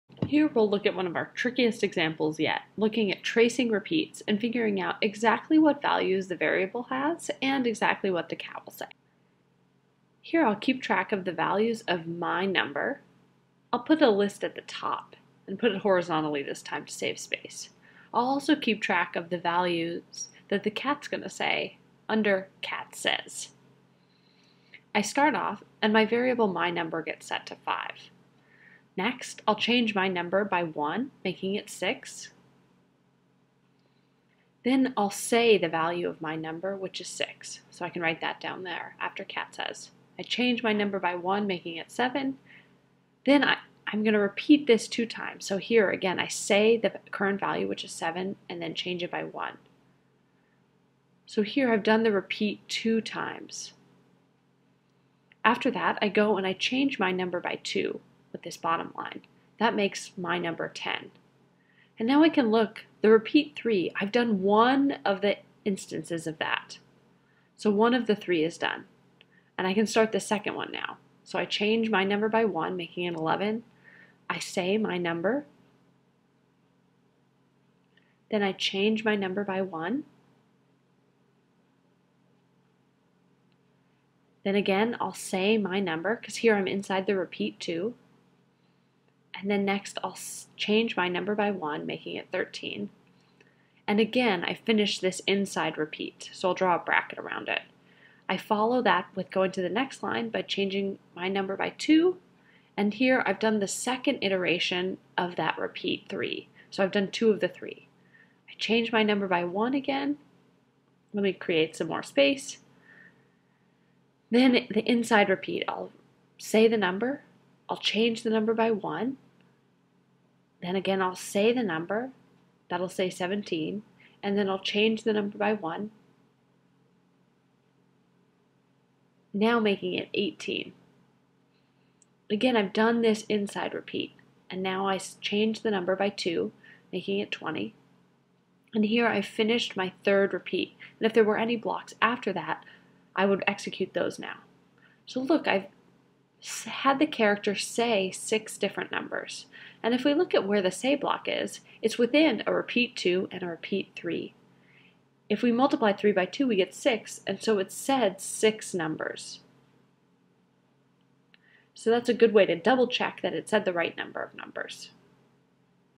Tracing Repeats #3: Programming in Scratch 2.0
It is helpful to keep a table of what the values of the variables are when you run a scripts. This shows a table for a difficult script.